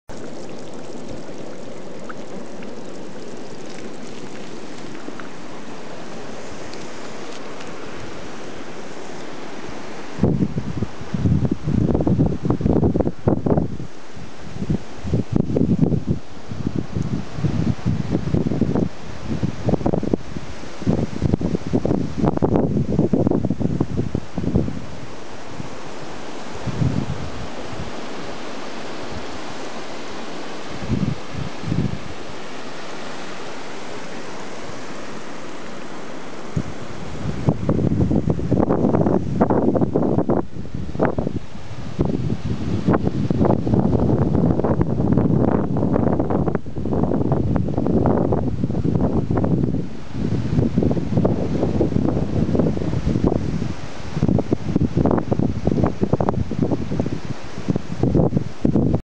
Минеральный источник. Даштыг-Хемский хребет, Восточные Саяны, Тыва
Исходное видео. Использовано в клипе про штурм перевала Салтасты во время трофи-экспедиции "Большой Саян-2010".
р. Аржан-Хем, Даштыг-Хемский хребет, Восточные Саяны, Тыва.
Водичка очень вкусная! Газированная.